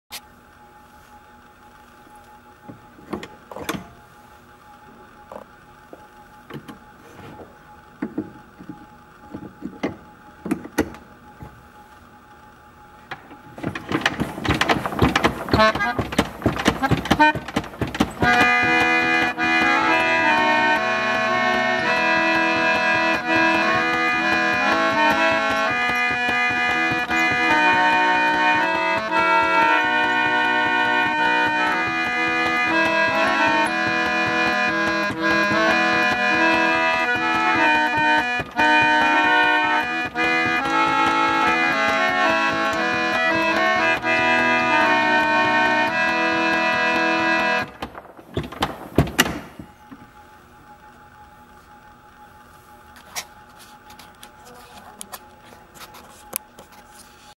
Victorian Walnut Concert Roller or Cob Organ, Crank Operated
A Victorian period roller or cob organ has a latest patent of 1887.

Ithaca, NY 1887 antique musical instrument
Signed "Autophone"
Works very well with hand crank for bellows
Walnut case
Original finish and original stenciling in very fine condition with minor wear
Original brass hardware
Glass door insert is wavy
Includes 11 cobs or rollers with hymns and popular tunes